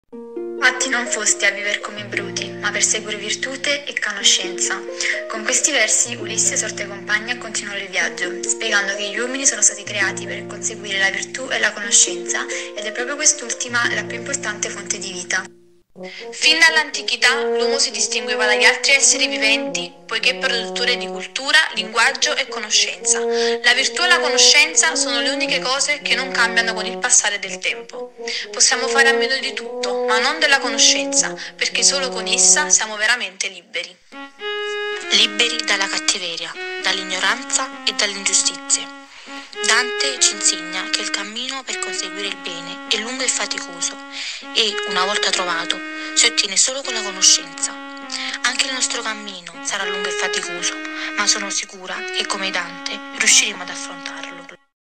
G.Migliaccio, S.Carandente, A.De Vivo. Dantedì🌿📕🇮🇹
Salve a tutti. Siamo tre ragazze dell'Istituto Magistrale Statale "Virgilio" di Pozzuoli(NA).
Questo è il nostro contributo al grande poeta Dante Alighieri.